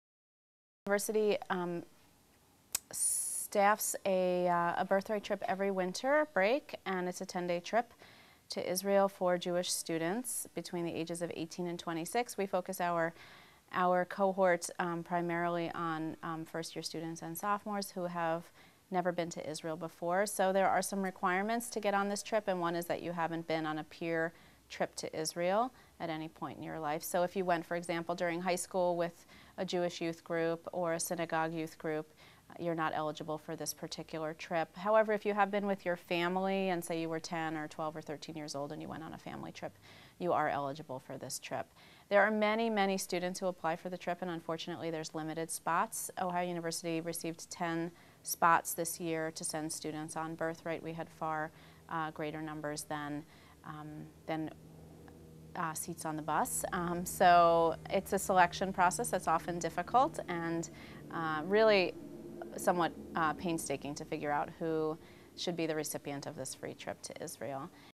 Athens MidDay News-Birthright Trip
Rabbi Danielle Leshaw talks about the Birthright trip to Israel.